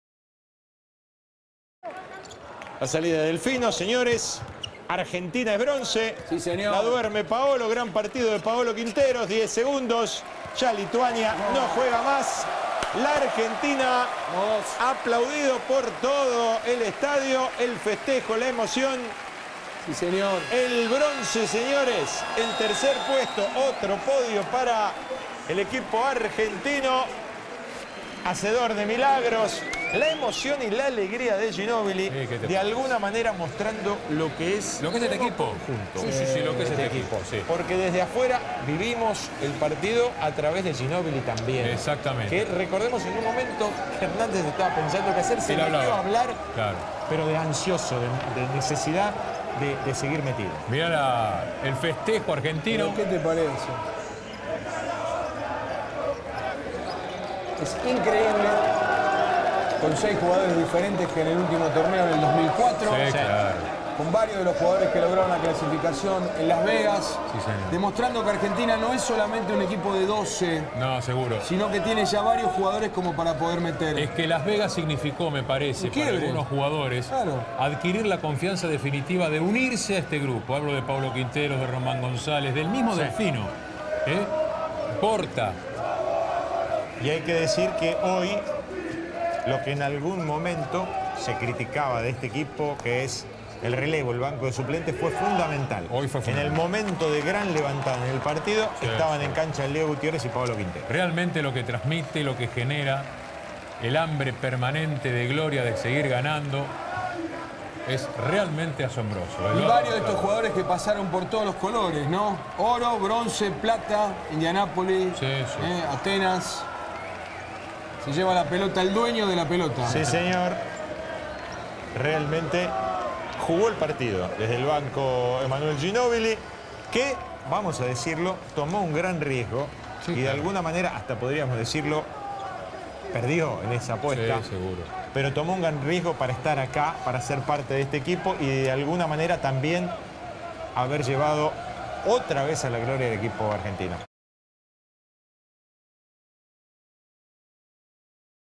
A 7 años de otro día de gloria: bronce ante Lituania en Pekín 2008
El partido se jugó a la mañana y Argentina estaba sin Ginóbili, lesionado en las semis ante Estados Unidos. Pero el coraje fue más grande que nunca y el triunfo permitió colgarse por segundo torneo seguido una medalla olímpica. Reviví el cierre del juego y los festejos.